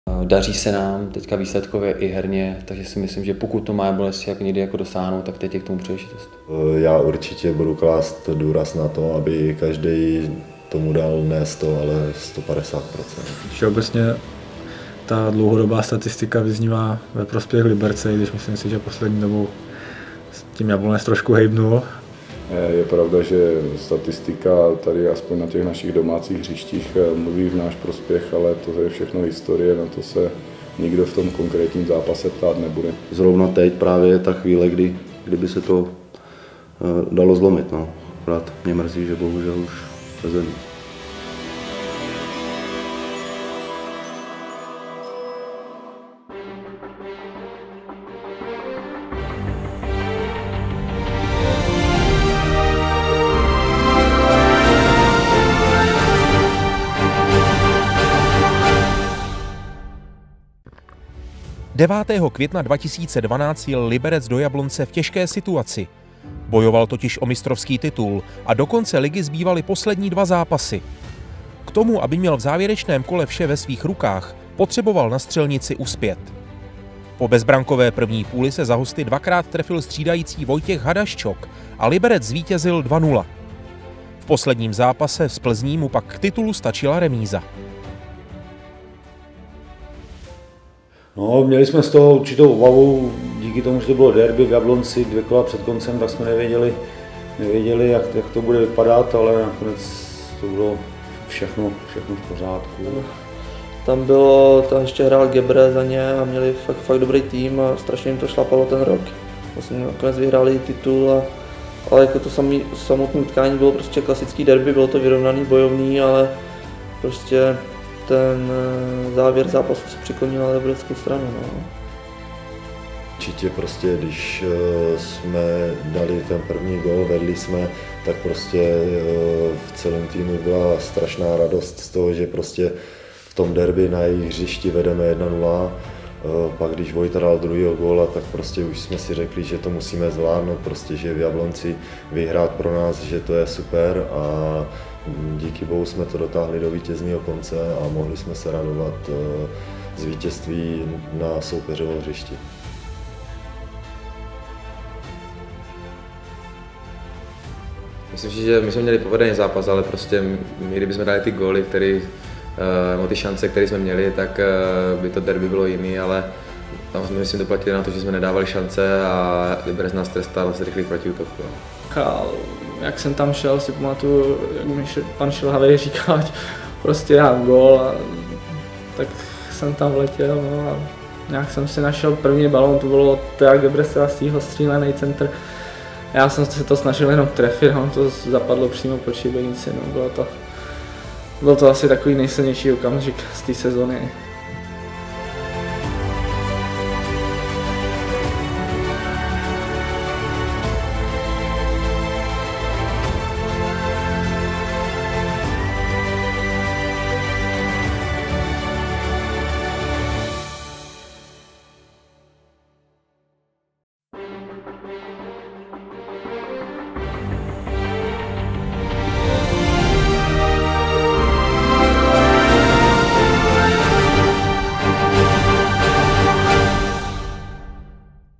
Podještědské derby: Jablonecká bojovnost
I když se zrovna nedařilo, tak jablonečtí fotbalisté vynikali vždy bojovností a houževnatostí. V roce 2012 se snažili přibrzdit Slovan na cestě za titulem. Loučkovi, Špitovi a Kopicovi se to nepodařilo. Tuto neděli to bude ale zcela jiný příběh. Ve videu také uvidíte: Hadaščoka, Lafatu, Zábojníka, Štajnera a další.